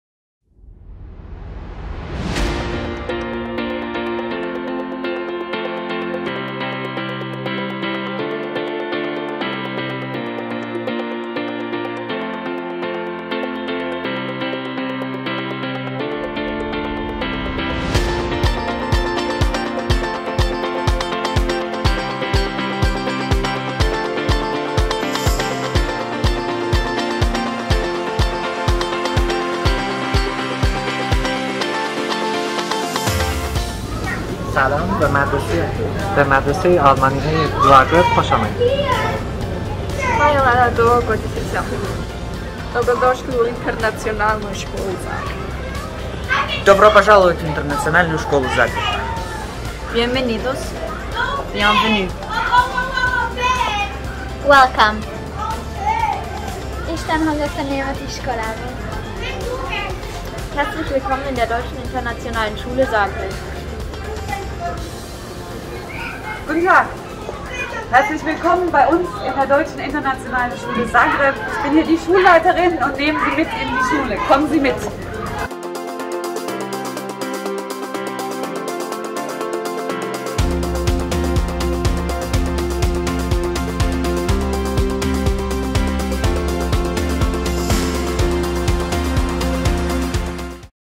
Njemačka međunarodna škola u Zagrebu (DISZ)
Dobro došli u Njemačku međunarodnu školu u Zagrebu!

Njemačka međunarodna škola u Zagrebu (DISZ) pripada mreži od 140 njemačkih škola u inozemstvu koje podupire Središnji ured za školstvo u inozemstvu (ZfA) po nalogu Ministarstva vanjskih poslova te koje su organizirane kao dobrotvorne udruge. Od Silicon Valleya do Sofia-e, od Shanghaia do Pariza njemačke škole u inozemstvu pridaju važnost atraktivnoj i izazovnoj ponudi koja se temelji na individualnom poticanju, višejezičnosti i susretu kultura. Njemački jezik i kultura, obrazovanje, susreti i razmjena čine uporište njemačkih škola u inozemstvu.